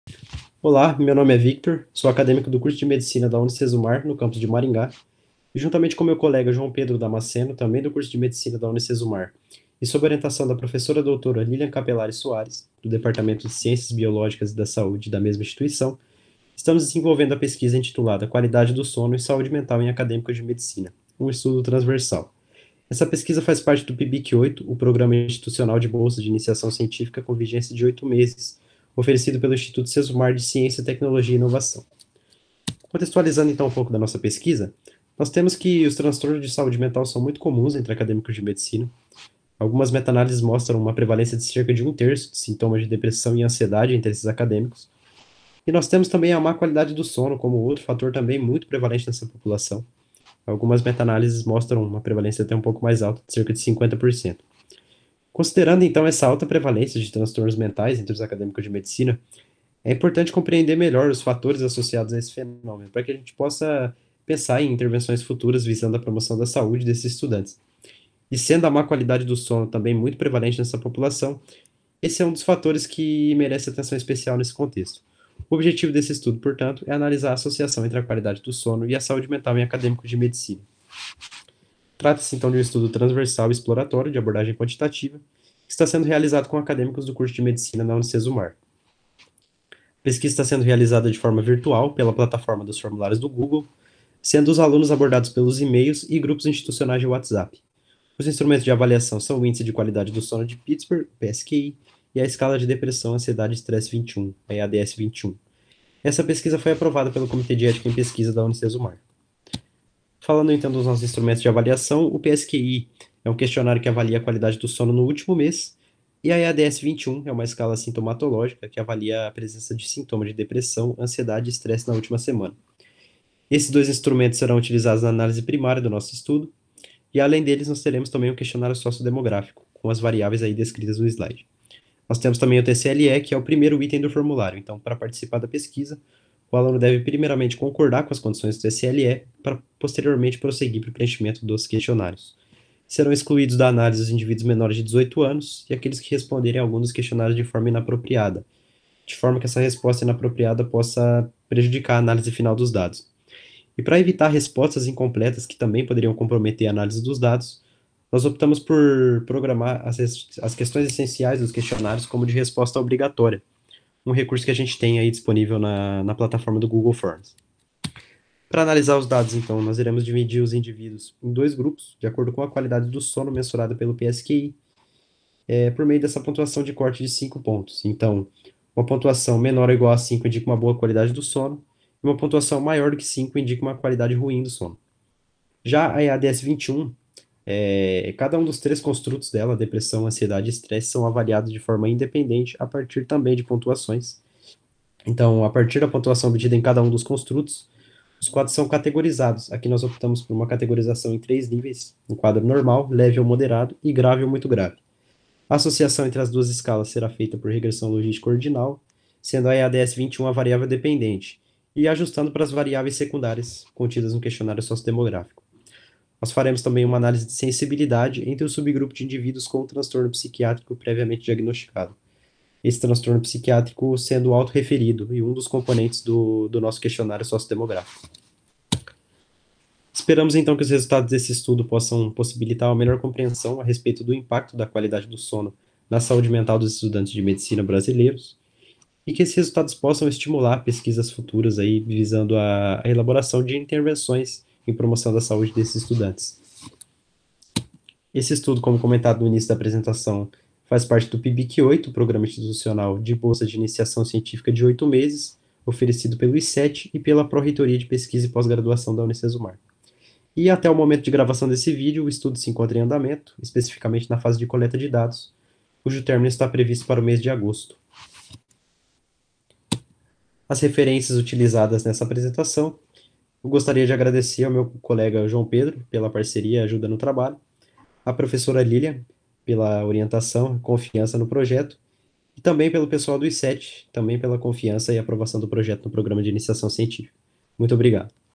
XII EPCC 2021 | Victor Perli - QUALIDADE DO SONO E SAÚDE MENTAL EM ACADÊMICOS DE MEDICINA: UM...
QUALIDADE DO SONO E SAÚDE MENTAL EM ACADÊMICOS DE MEDICINA: UM ESTUDO TRANSVERSAL